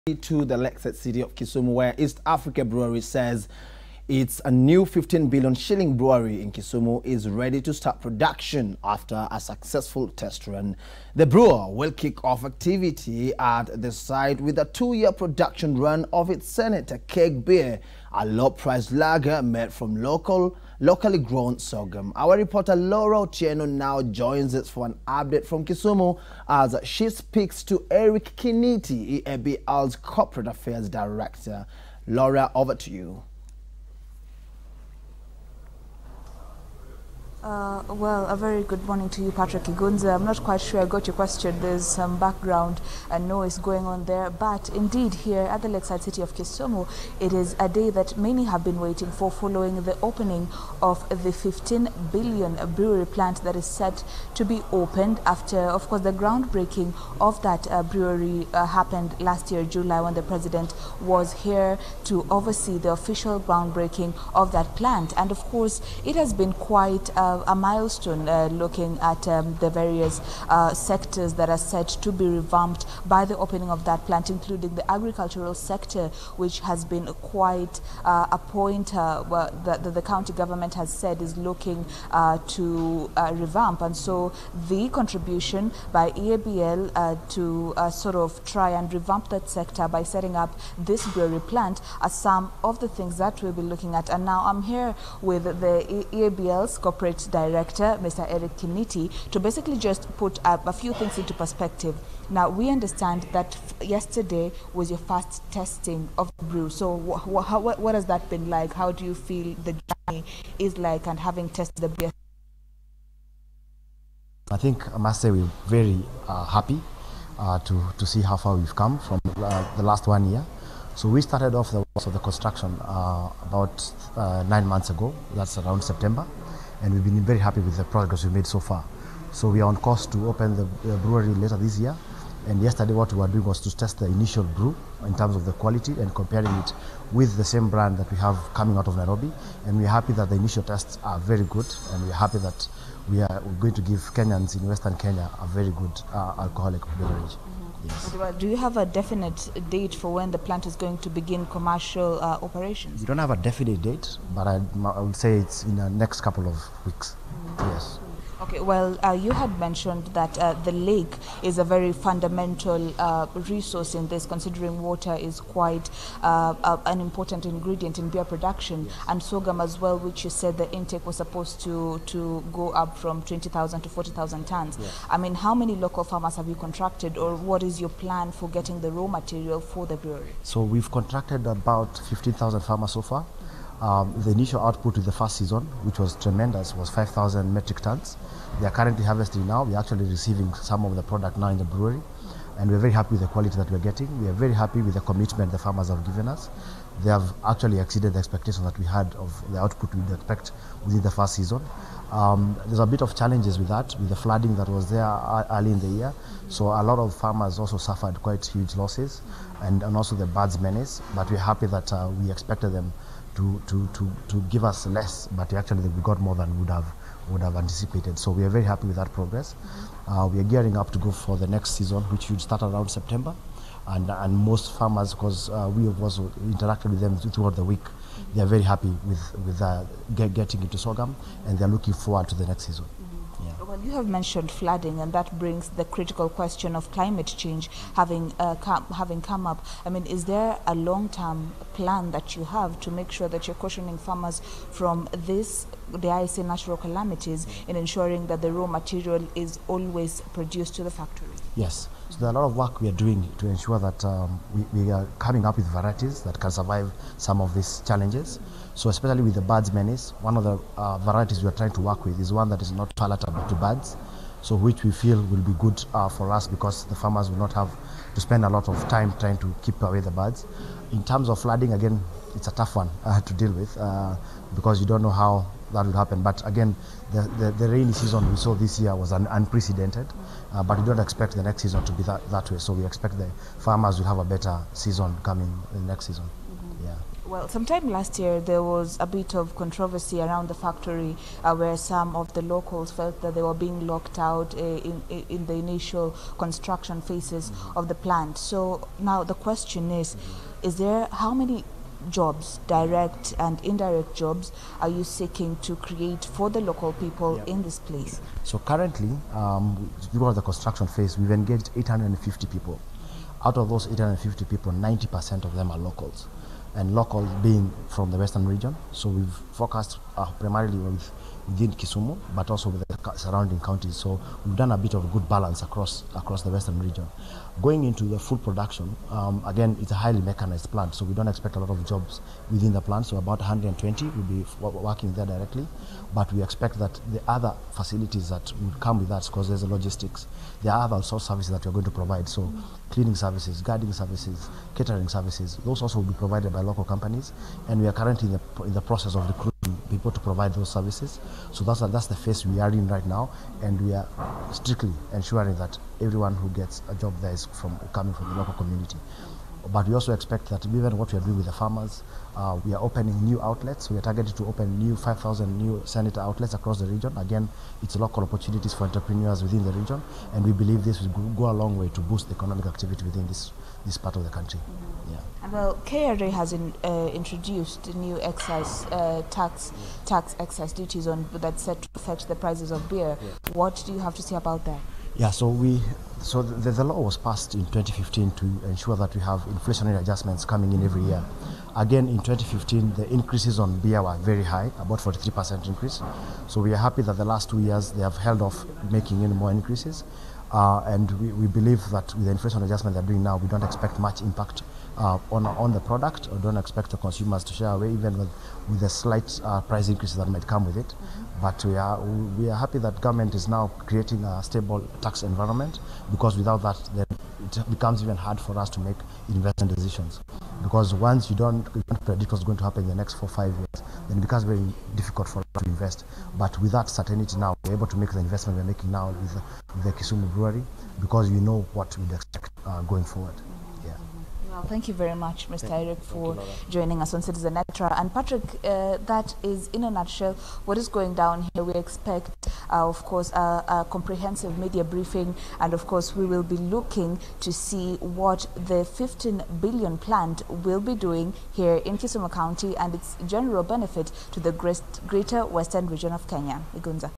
East Africa breweries 15b brewery ready to start
East African Breweries says its new 15 billion shilling brewery in Kisumu is ready to start production after a successful test run. The brewer will kick off activity at the site with a two-year production run of its Senator Keg beer, a low-priced lager made from locally grown sorghum.